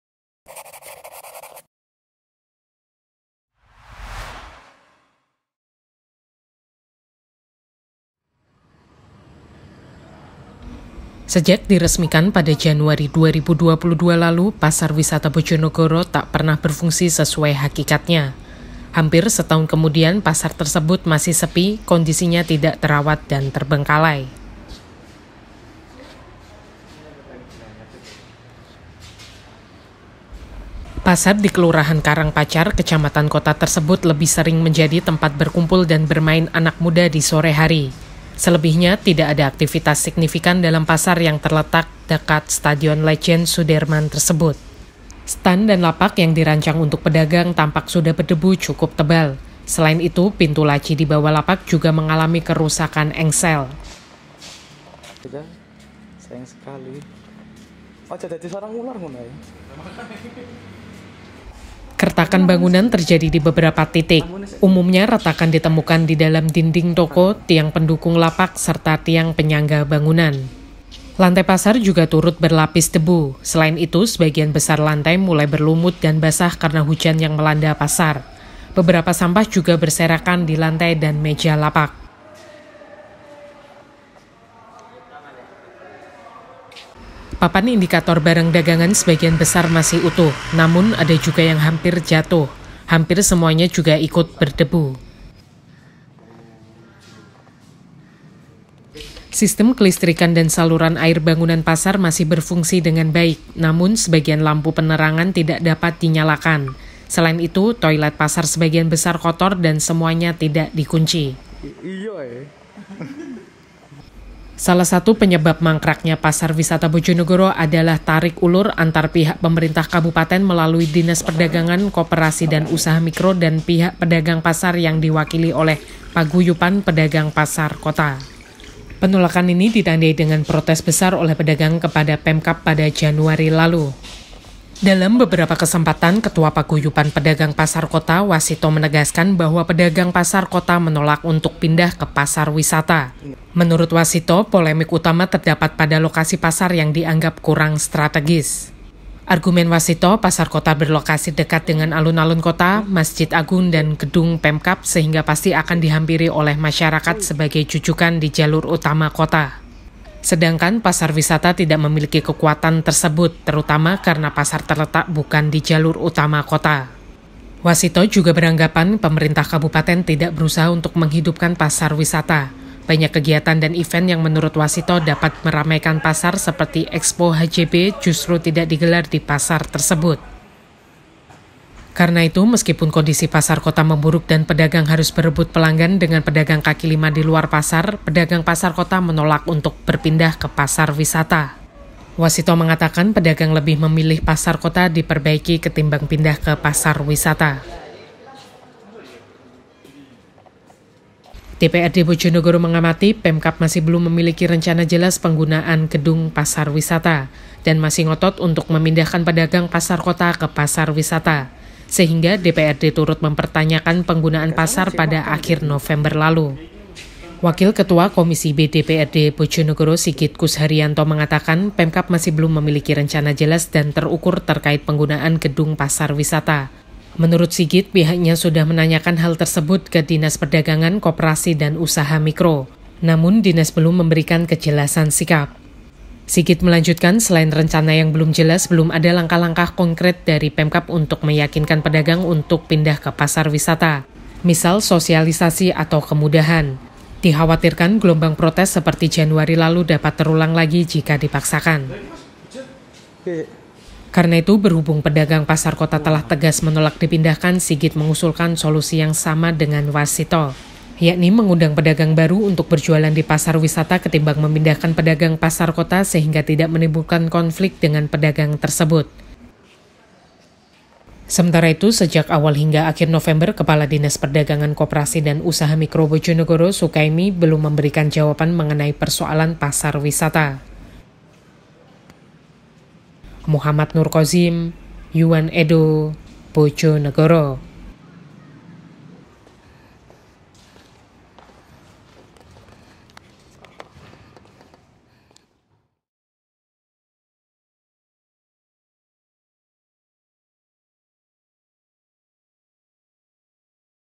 [BERITA RADAR] PASAR WISATA MANGKRAK HAMPIR SETAHUN
Sejak diresmikan pada Januari 2022 lalu, Pasar Wisata Bojonegoro tak pernah berfungsi sesuai hakikatnya. Hampir setahun kemudian, pasar tersebut masih sepi, kondisinya tidak terawat dan terbengkalai.

Pasar di Kelurahan Karangpacar, Kecamatan Kota tersebut lebih sering menjadi tempat berkumpul dan bermain anak muda di sore hari. Selebihnya, tidak ada aktivitas signifikan dalam pasar yang terletak dekat Stadion Letjen Sudirman tersebut.

Salah satu penyebab mangkraknya Pasar Wisata Bojonegoro adalah tarik-ulur antara pihak Pemerintah Kabupaten melalui Dinas Perdagangan, Koperasi, dan Usaha Mikro, dan pihak pedagang pasar yang diwakili oleh Paguyuban Pedagang Pasar Kota. Penolakan ini ditandai dengan protes besar oleh pedagang kepada pemkab pada Januari lalu.

Dalam beberapa kesempatan, Ketua Paguyuban Pedagang Pasar Kota, Wasito menegaskan bahwa pedagang Pasar Kota menolak untuk pindah ke Pasar Wisata. Menurut Wasito, polemik utama terdapat pada lokasi pasar yang dianggap kurang strategis.